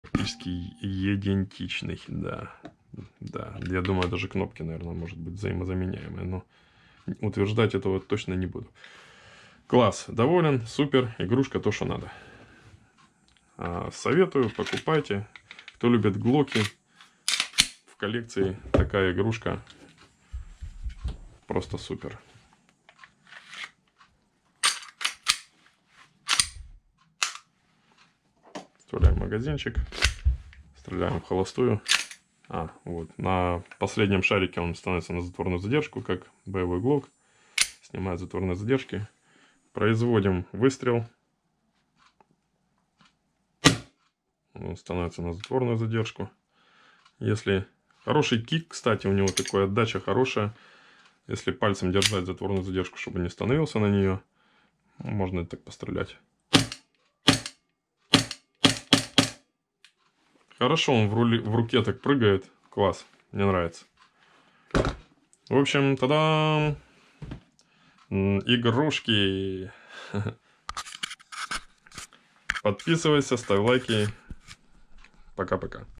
Umarex airsoft Glock 19x vs Real steel Glock 19x made in USA Russian language (part two)
Comparison Glock 19x airsoft vs real steel